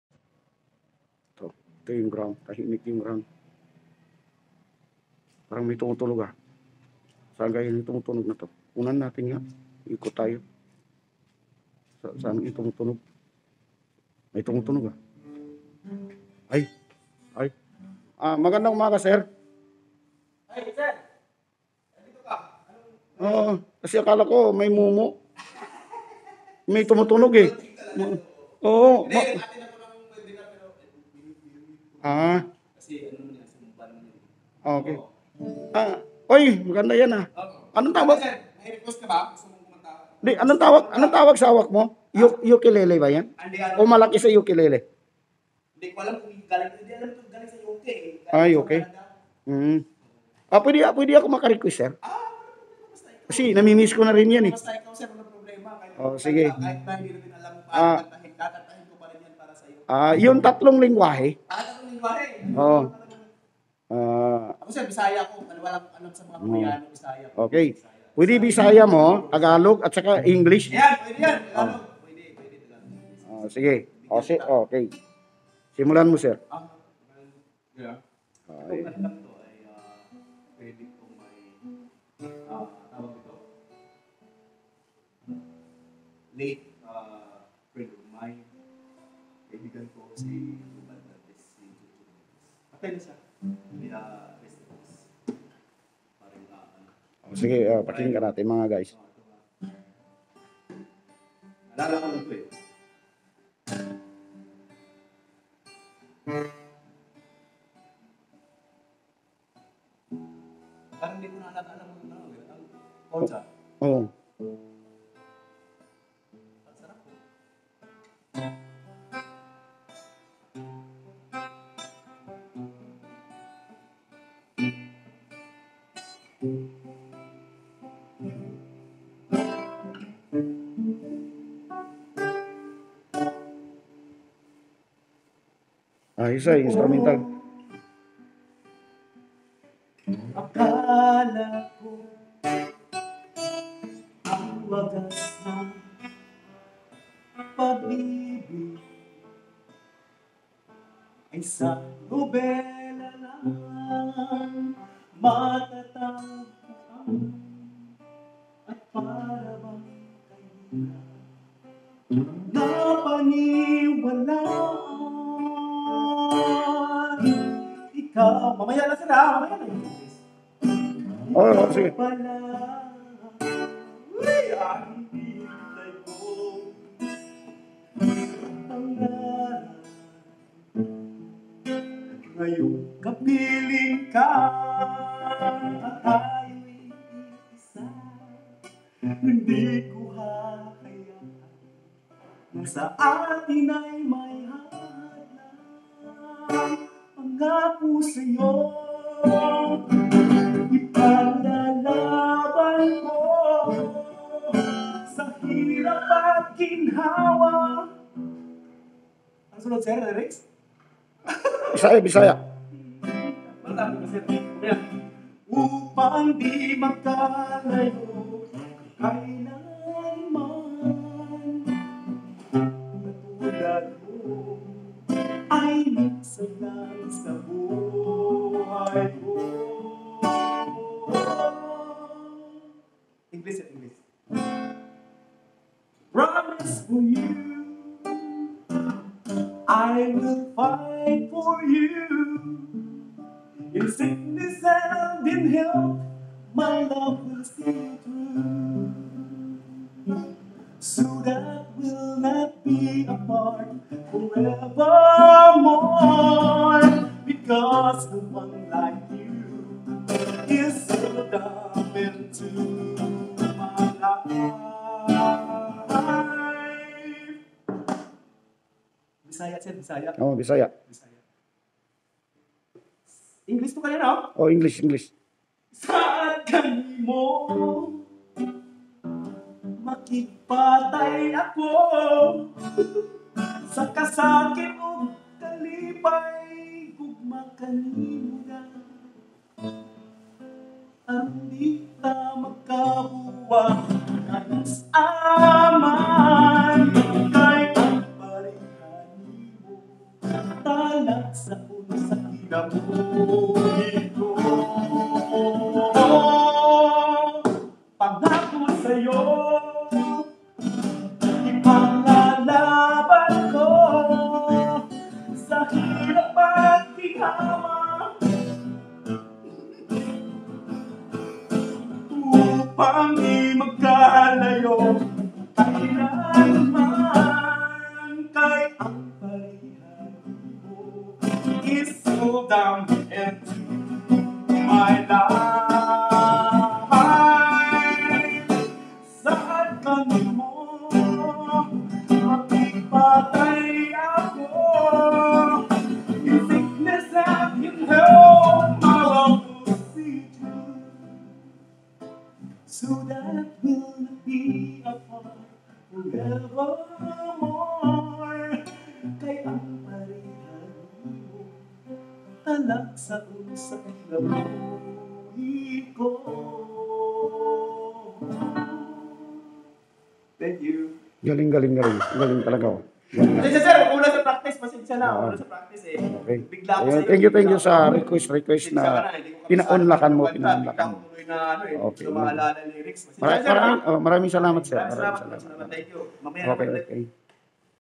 Ang tatlong wika na pinagsama-sama sa isang awit ni Sir Dan
mapapanood ninyo ang skills ng singer na ito na pinagsama sama ang tatlong wika